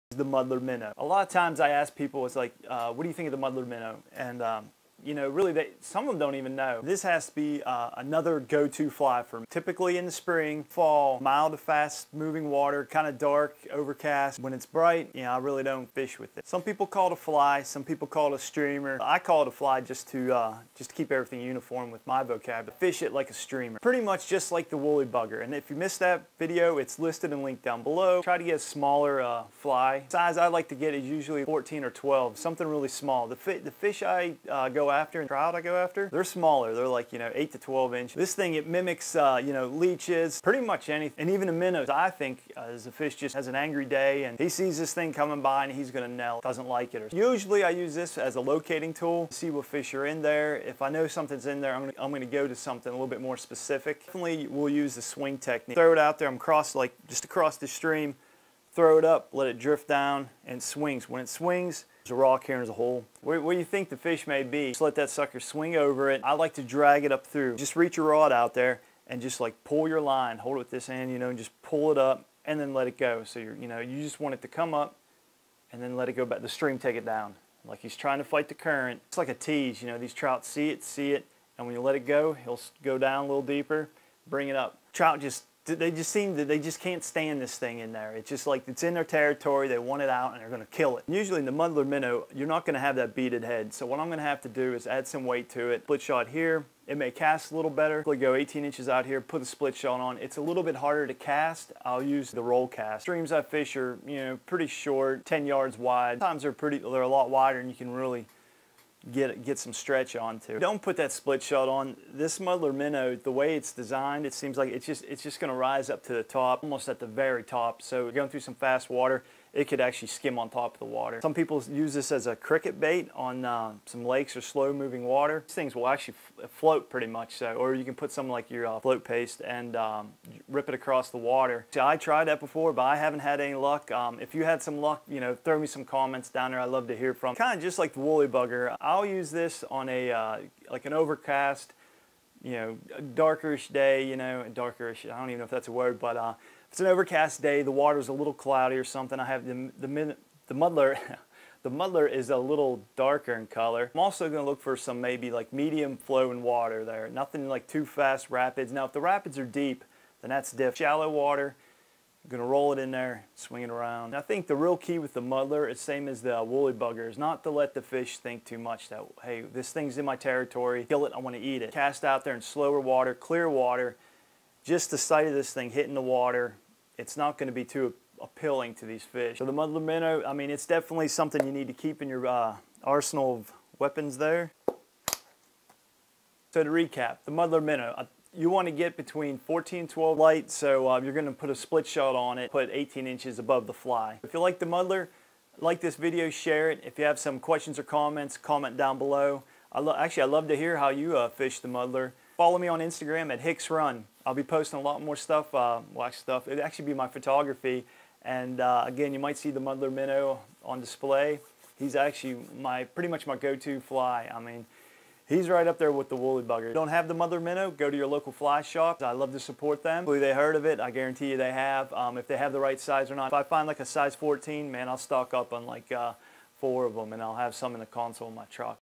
How I fish the Muddler Minnow
Outdoor enthusiast, Ken Hicks of Hicks Run discusses the muddler minnow.  Known to be his top secret fly and one of the most versatile. You can not go wrong with this one.  To Ken the muddler looks nothing like a minnow.
Ken fishes throws the muddler when you can’t see the fish and the water is moving.  If the water is slow and you see the fish, forget about it.  Flip rocks and match your fly to the bugs you find and sneak in on those fish.
The muddler is a hard fly to find.  If you can find a size 14, snatch up 4!  Add a split shot to get the muddler down deep, Ken places the split shot about 18 inches in front of the muddler.  Size and location of that weight will determine how the muddler casts and swims.
Kens favorite cast technique for the muddler or any streamer is the roll cast and let the muddler swing past the fish.  Tease the fish by bringing the muddler up the hole then let the line go so the muddler has to start at the beginning of the hole.  Bring him back through the hole like he is darting or nervous.  Trout hate this and either want to eat the muddler or just kill him to get him out of the hole.
The muddler minnow is one of the easiest flys to use.  You can’t screw this one up.

Tuesdays:  Adventure Photography
Thursdays: Fishing